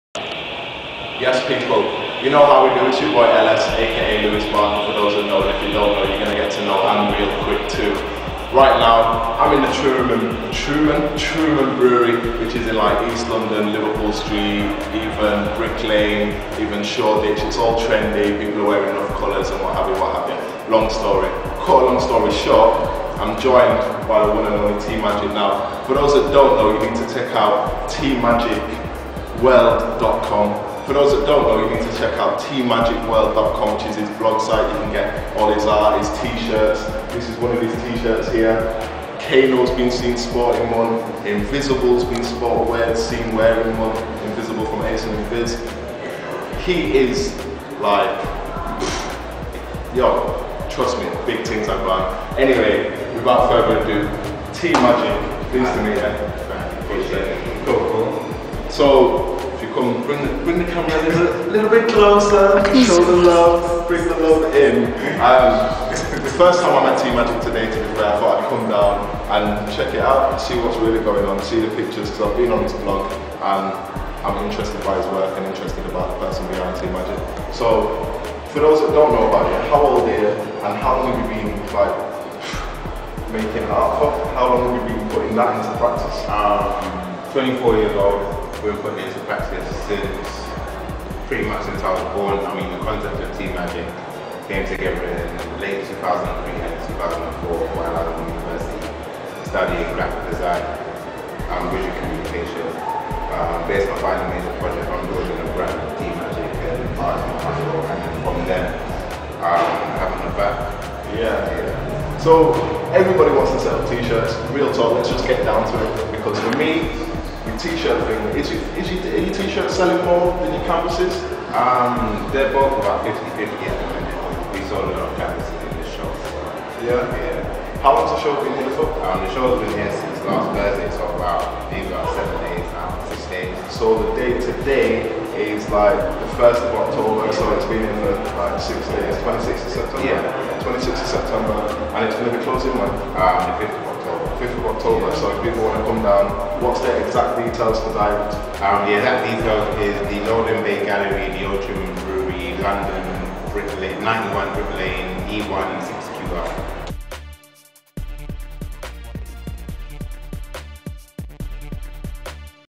Lewis Barker interviews T Magic
Host Lewis Barker catches up with artist T Magic at his loaded with meaning event at the Old Truman Brewery on Brick Lane. This is a short cut from the start of the interview and you may still have time to catch the event which is well worth it.